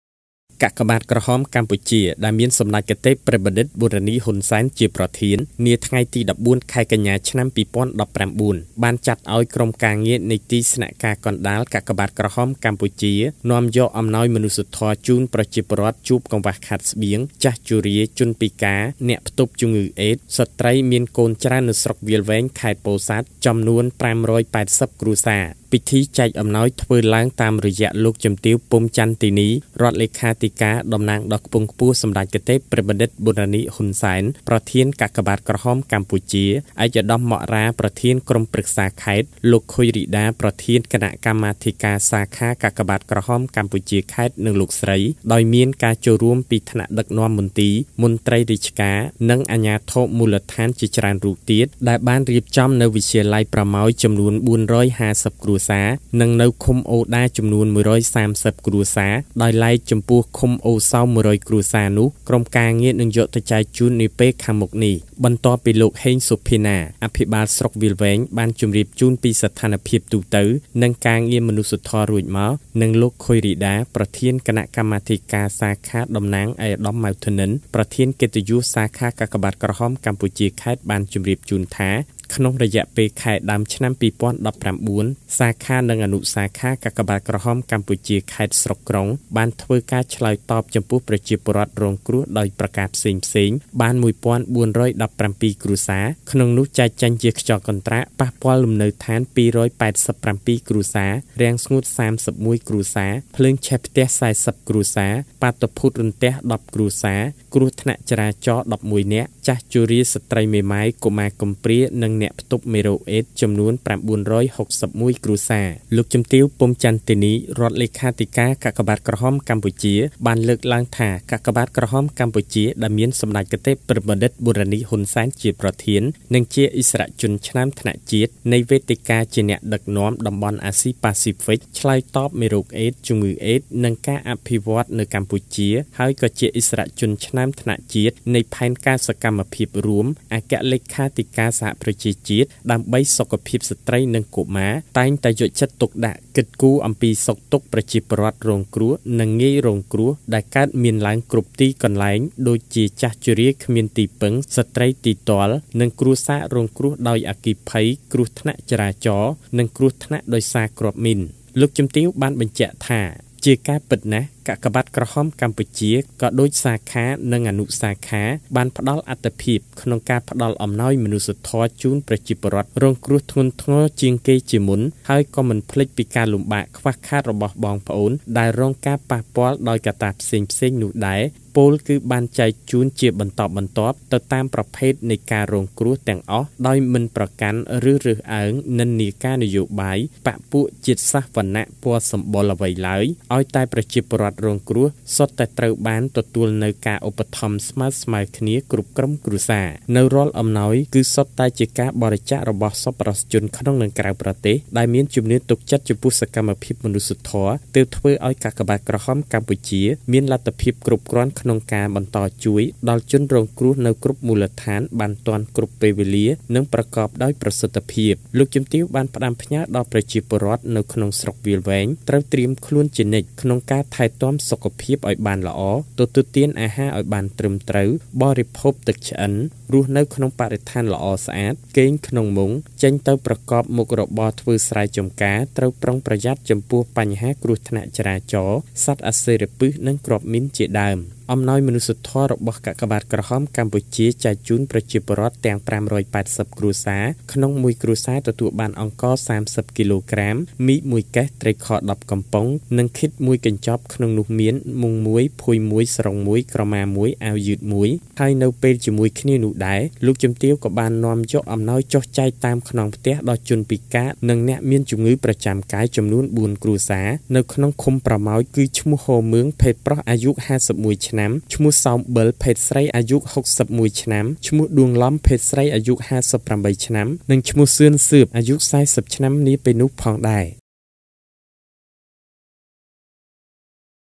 អំណោយមនុស្សធម៌របស់សាខាកាកបាទក្រហមកម្ពុជា ជូនប្រជាពលរដ្ឋនៅស្រុកវាលវែង៥៨០ គ្រួសារ
កាកបាទក្រហមកម្ពុជាដែលមានសម្តេចកិត្តិព្រឹទ្ធបណ្ឌិត ប៊ុន រ៉ានី ហ៊ុន សែន ជាប្រធាន នាថ្ងៃទី១៤ ខែកញ្ញា ឆ្នាំ២០១៩ បានចាត់ឲ្យក្រុមការងារនៃទីស្នាក់ការកណ្តាលកាកបាទក្រហមកម្ពុជា នាំយកអំណោយមនុស្សធម៌ជូនប្រជាពលរដ្ឋ ជួបកង្វះខាតស្បៀង ចាស់ជរា ជនពិការ អ្នកផ្ទុកជម្ងឺអេដស៍និងស្ត្រីមានកូនច្រើននៅស្រុកវាលវែង ខេត្តពោធិ៍សាត់ ចំនួន៥៨០គ្រួសារ ។ ពិធីចុះចែកអំណោយគឺធ្វើឡើងតាមរយៈ លោកជំទាវ ពុំ ចន្ទីនី អគ្គលេខាធិការ តំណាងដ៏ខ្ពង់ខ្ពស់សម្តេចកិត្តិព្រឹទ្ធបណ្ឌិត ប៊ុន រ៉ានី ហ៊ុន សែន ប្រធានកាក បាទក្រហមកម្ពុជា ឯកឧត្តម ម៉ក់ រ៉ា ប្រធាន ក្រុមប្រឹក្សាខេត្ត លោក ខូយ រីដា ប្រធានគណកម្មាធិការសាខាកាកបាទក្រហមកម្ពុជាខេត្តនិងលោកស្រី ដោយមានការចូល រួមពី ថ្នាក់ដឹកនាំមន្ទីរ មន្ត្រីរាជការ និងអាជ្ញាធរមូលដ្ឋានជាច្រើនទៀត ដែលបានរៀបចំនៅវិទ្យាល័យប្រម៉ោយចំនួន៤៥០គ្រួសារ និងនៅឃុំថ្មដាចំនួន១៣០ គ្រួសារ ដោយឡែកចំពោះឃុំអូរសោម១០០គ្រួសារនោះក្រុមការងារនឹងយកទៅចែកជូននាពេលខាងមុខនេះ។
 បន្ទាប់ពីលោក ហេង សុភាណា អភិបាលស្រុកវាលវែង បានជម្រាបជូនស្ថានភាពទូទៅនិងការងារមនុស្សធម៌នោះរួចមក និងលោកខូយ រីដា ប្រធានគណកម្មាធិការសាខាតំណាង ឯកឧត្តម ម៉ៅ ធនិន ប្រធានកិត្តិយសសាខាកាកបាទក្រហមកម្ពុជាខេត្តបានជម្រាបជូនថា ក្នុងរយៈពេលខែដើមឆ្នាំ២០១៩ សាខានិងអនុសាខា កាកបាទក្រហមកម្ពុជាខេត្ត ស្រុក ក្រុង បានធ្វើការឆ្លើយតបចំពោះ ប្រជា ពលរដ្ឋ រងគ្រោះដោយប្រការផ្សេងៗបាន ១៤១៧គ្រួសារក្នុងនោះចែកចេញជាខ្យល់កន្ត្រាក់ប៉ះពាល់លំនៅដ្ឋាន ២៨៧គ្រួសារ រាំងស្ងួត ៣១គ្រួសារភ្លើងឆេះផ្ទះ ៤០គ្រួ សារ បាតុភូតរន្ទះ១០នាក់ គ្រោះថ្នាក់ចរាចរណ៍១១នាក់ ចាស់ជរា ស្ត្រីមេម៉ាយ កុមារកំព្រានិងអ្នកផ្ទុកមេរោគអេដស៍ចំនួន៩៦១គ្រួសារ ។
            លោកជំទាវ ពុំ ចន្ទីនី អគ្គលេខាធិការ កាកបាទក្រហមកម្ពុជា បានលើកឡើងថា កាកបាទក្រហមកម្ពុជា ដែលមានសម្តេច កិត្តិព្រឹទ្ធ បណ្ឌិត ប៊ុន រ៉ានី ហ៊ុន សែន ជាប្រធាន និងជាឥស្សរជនឆ្នើមថ្នាក់ជាតិនៃវេទិកាភាពជាអ្នកដឹកនាំ តំបន់អាស៊ី បាស៊ីហ្វិក ឆ្លើយតប មេរោគអេដស៍ ជម្ងឺអេដស៍និងការអភិវឌ្ឍន៍នៅកម្ពុជា ហើយក៏ជា ឥស្សរជនឆ្នើម ថ្នាក់ជាតិនៃផែនការសកម្មភាពរួម អគ្គលេខាធិការអង្គការសហប្រជាជាតិ ដើម្បីសុខភាពស្ត្រីនិងកុមារ តែងតែយកចិត្តទុកដាក់ គិតគូ អំពីសុខទុក្ខប្រជាពលរដ្ឋ រងគ្រោះនិងងាយរងគ្រោះដែលកើតមានឡើង នៅគ្រប់ទី កន្លែង ដូចជា ចាស់ជរាគ្មានទីពឹង ស្ត្រីទីទ័ល និងគ្រួសាររងគ្រោះដោយអគ្គិភ័យ គ្រោះថ្នាក់ចរាចរណ៍ និងគ្រោះថ្នាក់ដោយសារគ្រាប់មីន។ លោកជំទាវ បានបញ្ជាក់ថា ជាការពិត កាកបាទក្រហមកម្ពុជា ក៏ដូចសាខានិងអនុសាខា បានផ្តល់អាទិភាព ក្នុងការផ្តល់អំណោយ មនុស្សធម៌ដល់ប្រជាពលរដ្ឋ រងគ្រោះដែលធ្ងន់ធ្ងជាងគេជាមុន ហើយក៏មិនភ្លេច ពីការលំបាកខ្វះខាត របស់បងប្អូន ដែលរងការប៉ះពាល់ ដោយកត្តាផ្សេងៗ នោះដែរ ពោលគឺ  បានចែកជូនជាបន្តបន្ទាប់ ទៅតាមប្រភេទនៃការរងគ្រោះទាំងអស់ដោយមិនបានប្រកាន់ ឬរើសអើង ពីនិន្នាការ នយោបាយ បក្ខពួក ជាតិ សាសន៍ វណ្ណៈ ព៌ណសម្បុរ អ្វីឡើយឱ្យតែប្រជាពលរដ្ឋរងគ្រោះ សុទ្ធតែត្រូវបានទទួល នូវការឧបត្ថម្ភ ស្មើៗគ្នា គ្រប់ក្រុមគ្រួសារ ។ នូវរាល់អំណោយ គឺសុទ្ធតែជាការបរិច្ចាករបស់សប្បុរសជន ក្នុងនិងក្រៅប្រទេស ដែលមានជំនឿទុកចិត្តចំពោះសកម្មភាពមនុស្សធម៌ទើបធ្វើឲ្យកាកបាទក្រហមកម្ពុជា មានលទ្ធភាពគ្រប់គ្រាន់ក្នុងការ បន្តជួយ ដល់ជនរងគ្រោះនៅគ្រប់មូលដ្ឋាន បានទាន់ពេលវេលា និងប្រកប ដោយប្រសិទ្ធិភាព ។លោកជំទាវ បានផ្តាំផ្ញើដល់ប្រជាពលរដ្ឋនៅក្នុងស្រុកវាលវែង ត្រូវត្រៀមខ្លួន ជានិច្ច ក្នុងការថែទាំសុខភាព ឲ្យបានល្អ ទទួលទានអាហារឲ្យបានត្រឹមត្រូវ បរិភោគទឹកឆ្អិន រស់នៅក្នុងបរិស្ថានល្អស្អាត គេងក្នុងមុង ចេញទៅប្រកបមុខរបរធ្វើស្រែចំការ ត្រូវប្រុងប្រយ័ត្ន ចំពោះបញ្ហាគ្រោះថ្នាក់ចរាចរណ៍ សត្វអសិរពិស និងគ្រាប់មីនជាដើម ។ អំណោយមនុស្សធម៌របស់កាកបាទក្រហមកម្ពុជាចែកជូនប្រជាពលរដ្ឋទាំង៥៨០គ្រួសារដោយក្នុងមួយគ្រួសារទទួលបានអង្ករ៣០គីឡូក្រាម មី១កេស ត្រីខ១០កំប៉ុង និងឃីត១កញ្ចប់ក្នុងនោះមាន មុង១ ភួយ១ សារុង១ ក្រមា១និងអាវយឺត១ ហើយ នៅពេលជាមួយគ្នានោះដែរលោកជំទាវក៏បាននាំយកអំណោយចុះចែកតាមខ្នងផ្ទះដល់ជនពិការនិងមានជម្ងឺប្រចាំកាយ ចំនួន៤គ្រួសារ នៅក្នុងឃុំប្រម៉ោយ គឺ ឈ្មោះ ហ៊ មឿង ភេទប្រុស អាយុ ៥១ឆ្នាំ ឈ្មោះ សោម ប៉ិល ភេទស្រី អាយុ ៦១ឆ្នាំ ឈ្មោះ ដួង ឡំ ភេទស្រី អាយុ ៥៨ឆ្នាំ និងឈ្មោះ សឿន សឿម ភេទស្រី អាយុ ៤០ ឆ្នាំ ៕  ស្ងួន ឈឿន ពោធិ៍សាត់